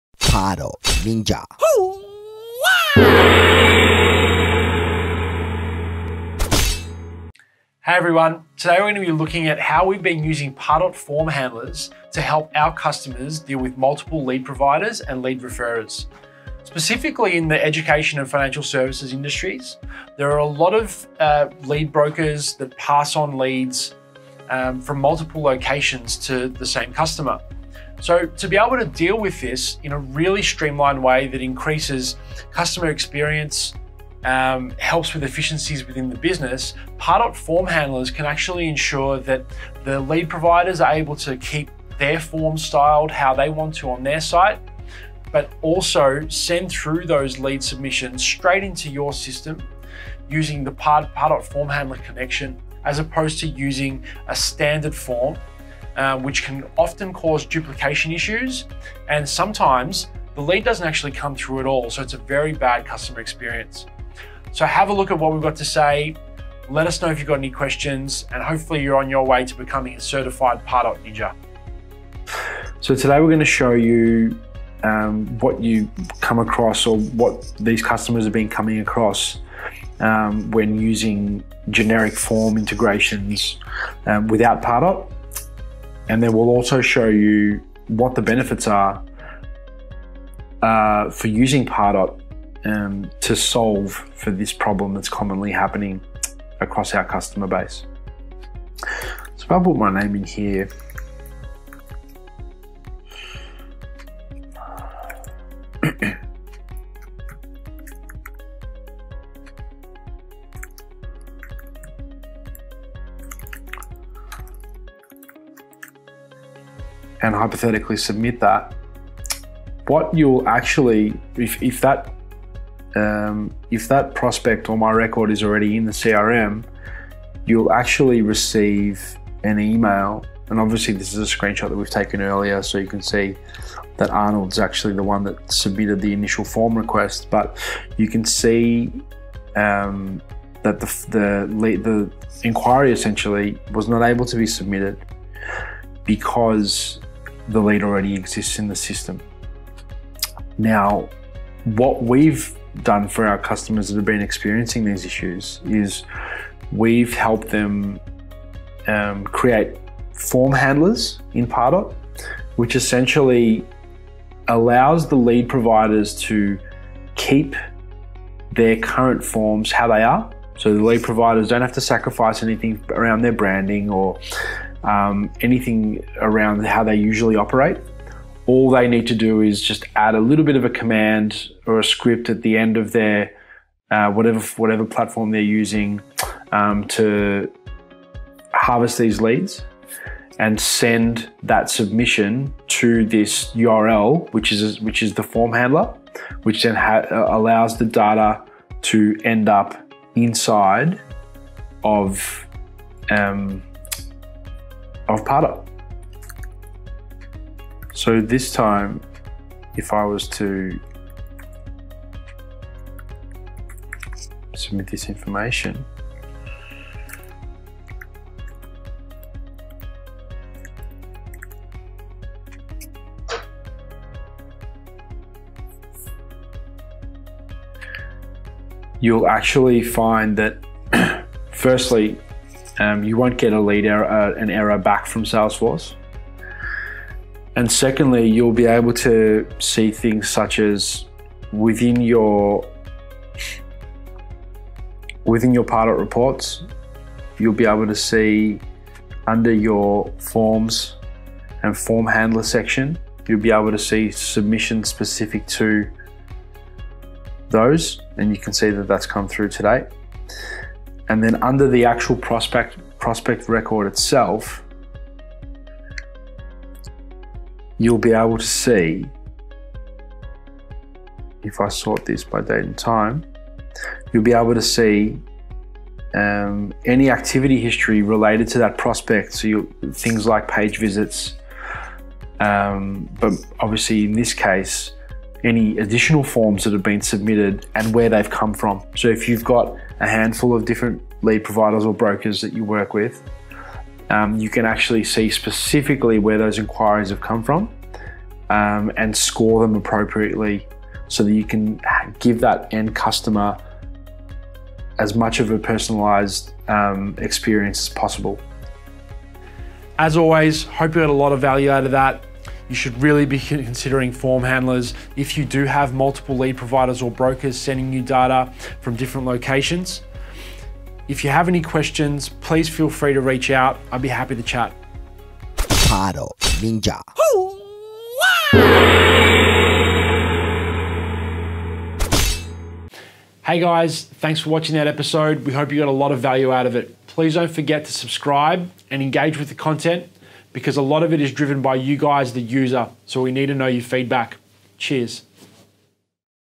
PARDOT FORM HANDLERS TUTORIAL | Lead Submissions from Multiple Lead Providers
Pardot Form Handlers are the perfect tool to help customers deal with multiple lead providers and lead referrers, sourcing leads from multiple locations for that same customer.

Using Pardot Form Handlers means:
✅Lead providers can maintain their own form styling on their own platforms, while still sending lead submissions straight into your system.
✅Avoid duplication issues or the lead submission not coming through at all.
✅Improve efficiency within your business and provide a seamless customer experience all round!